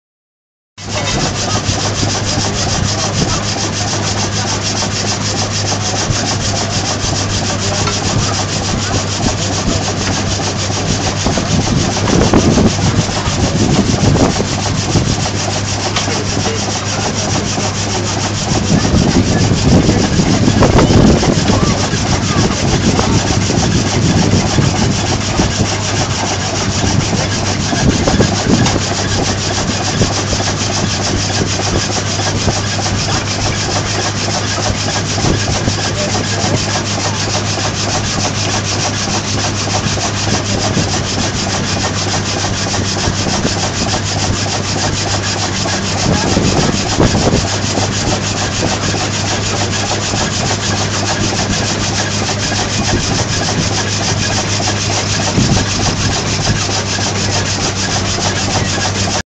oude mobiele houtzagerij.mp4
prachtige oude stoom houtzagerij, gefilmd tijdens de internationale oldtimerdagen in Tilligte zoooo mooi dat oude spul..
Steam Steamsaw